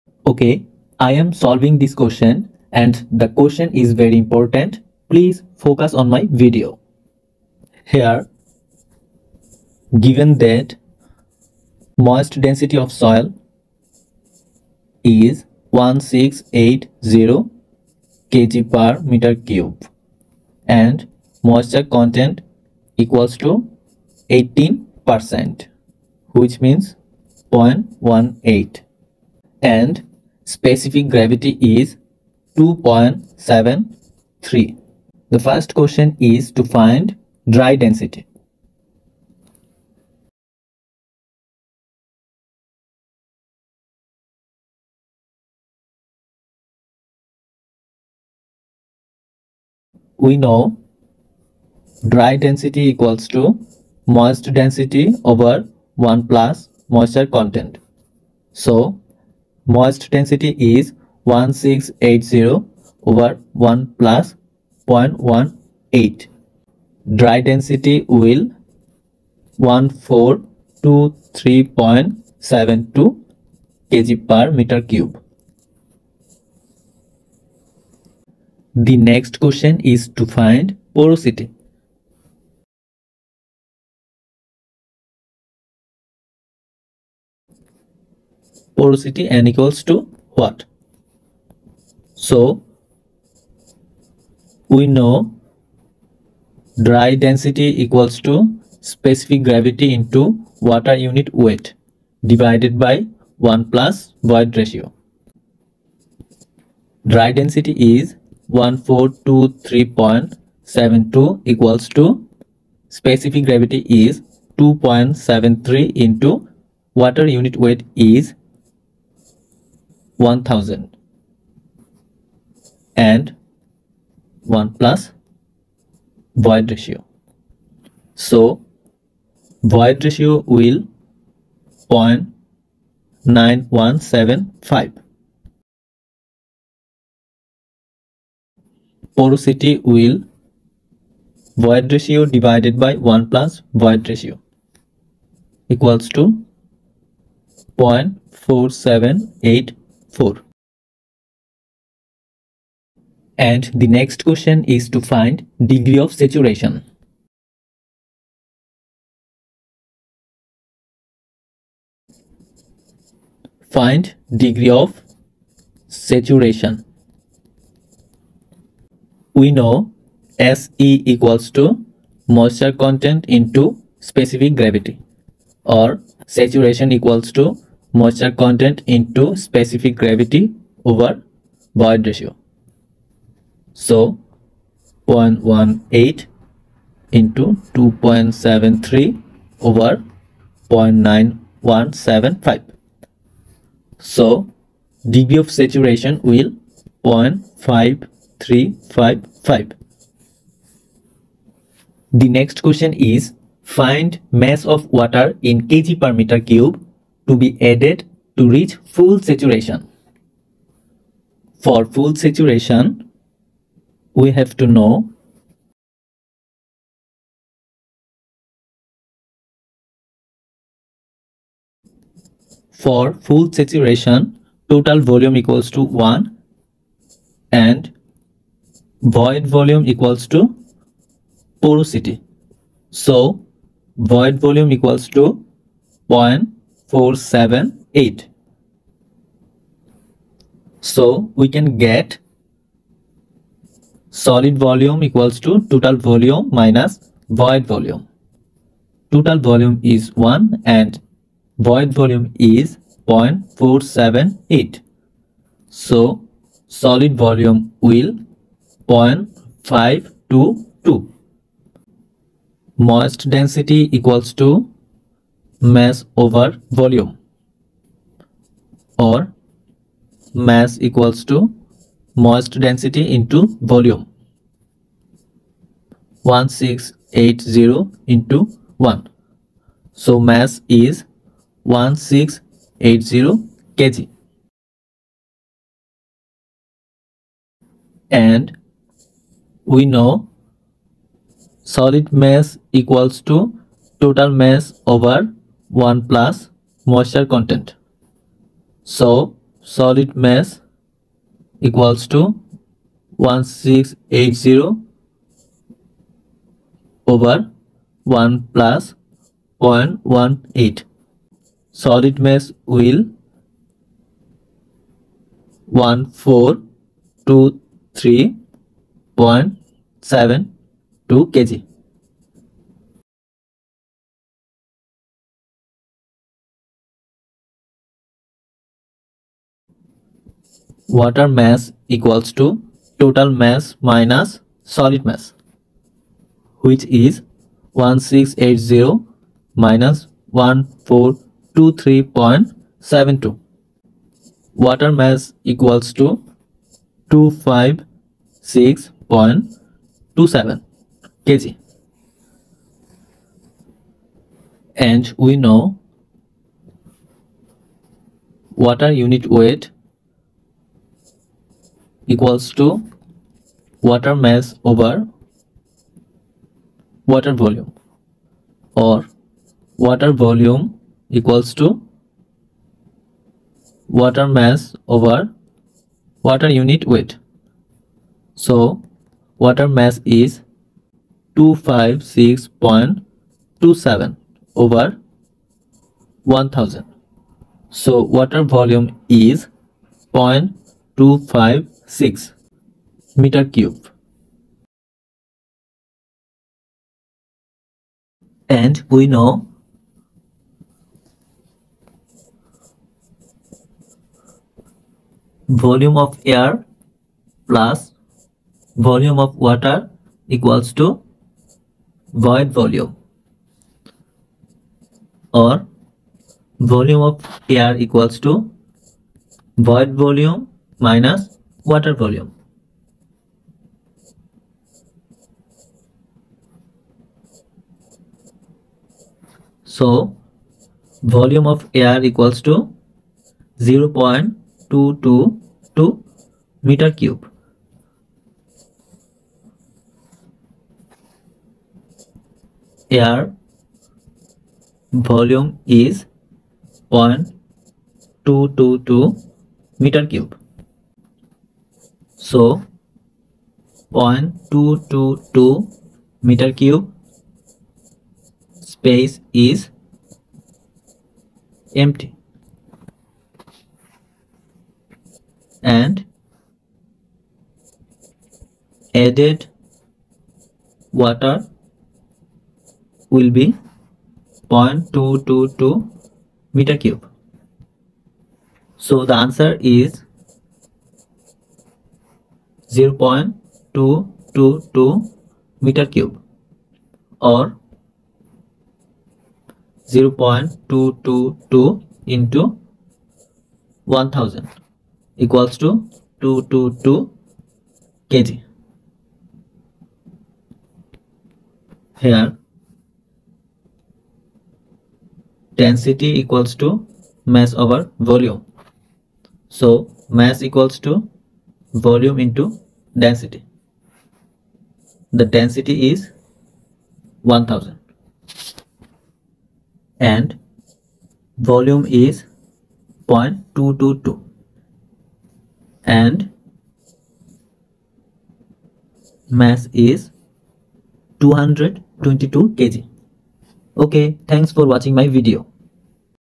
The moist density of soil is 1680 kg/m^3.Given w%=18%,Gs=2.73, determine mass of water in kg/m^3
The moist density of a soil is 1680 kg/m^3. Given  w%=18% and Gs=2.73,  determine mass of water in kg/m^3   dry density, porosity degree of saturation,